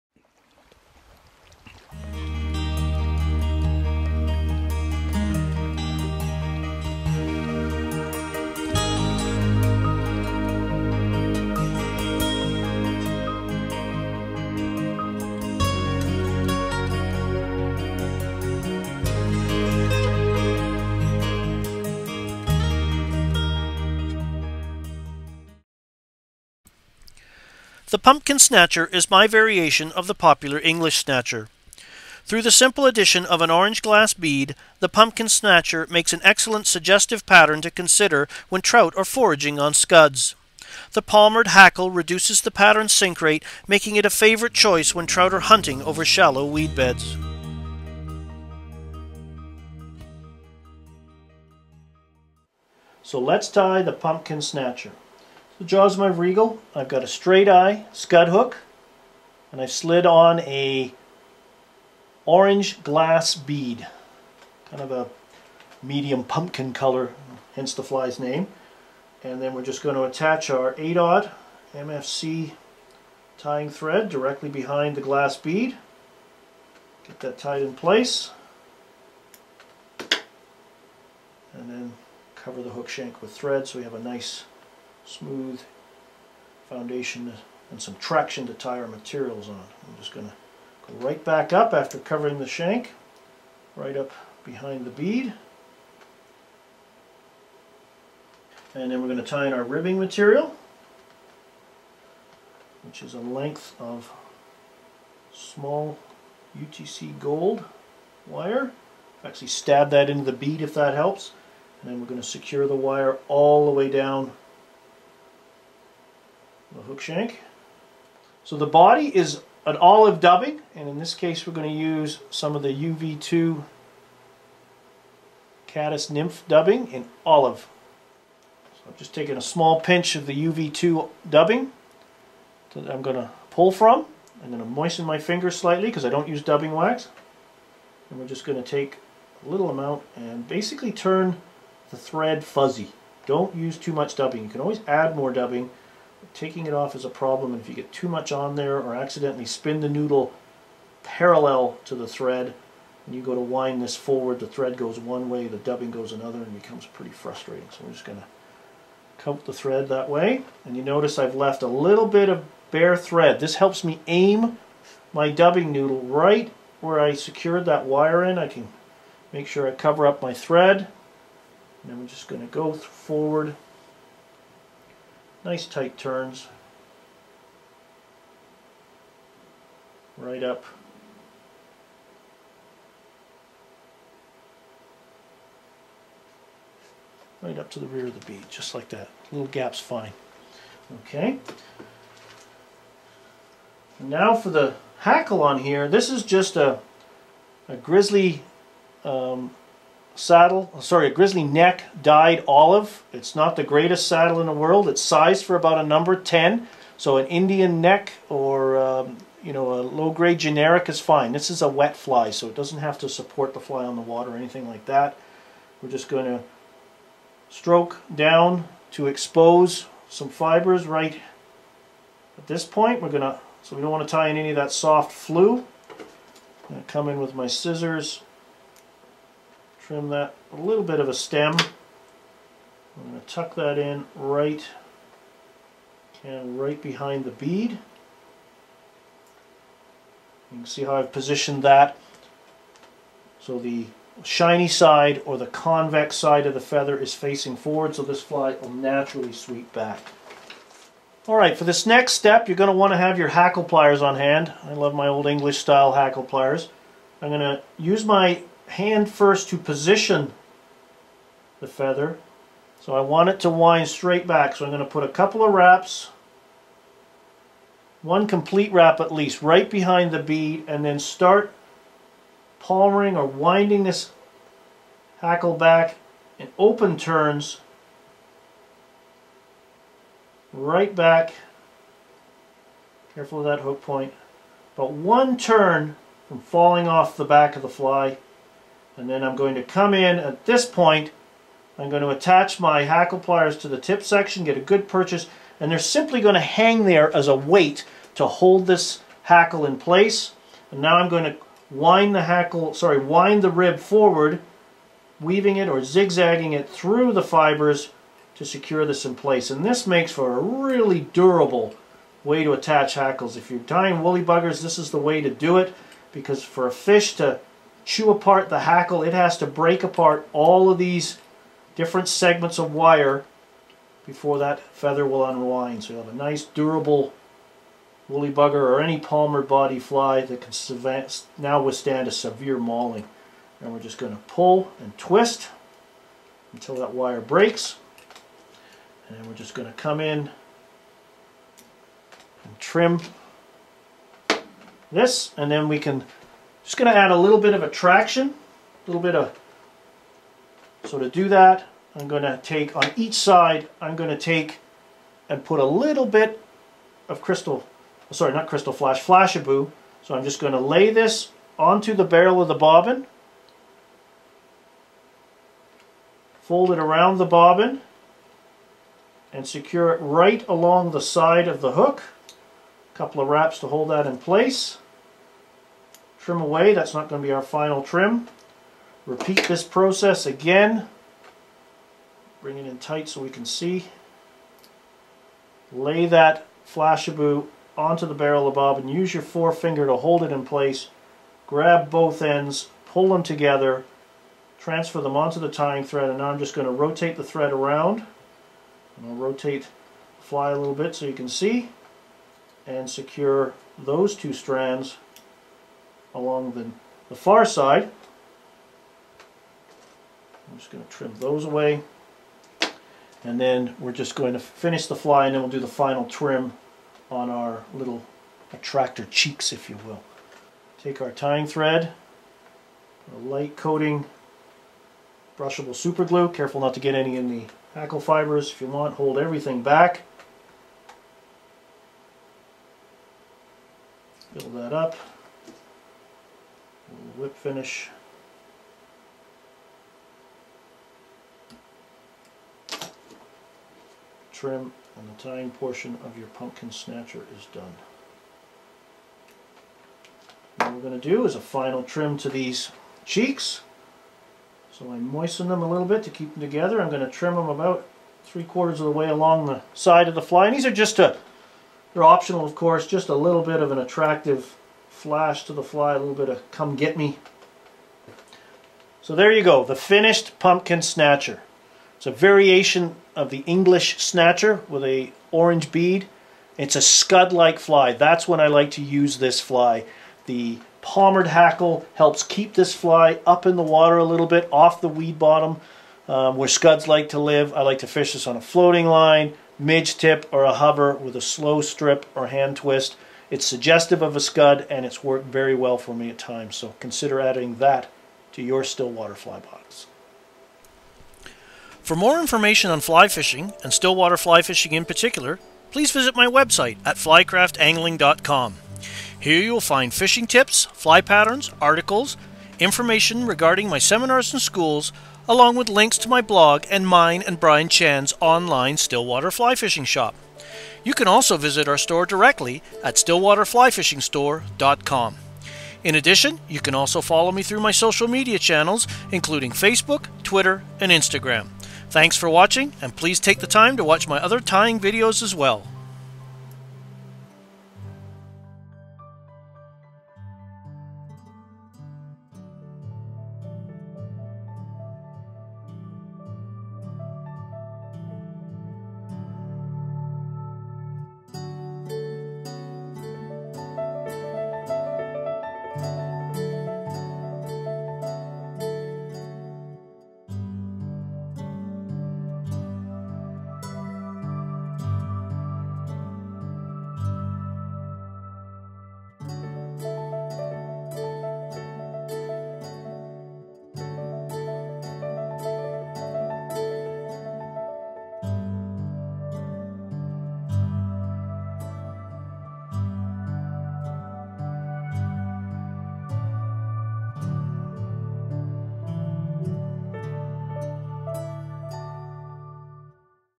Pumpkin Snatcher
Scuds are a stillwater staple and when present in large concentrations they are a steady staple for stillwater trout.  A healthy influx of scuds in the diet allows trout to pack on the pounds.  The Pumpkin Snatcher is a suggestive pattern that works well when trout become fixated on scuds.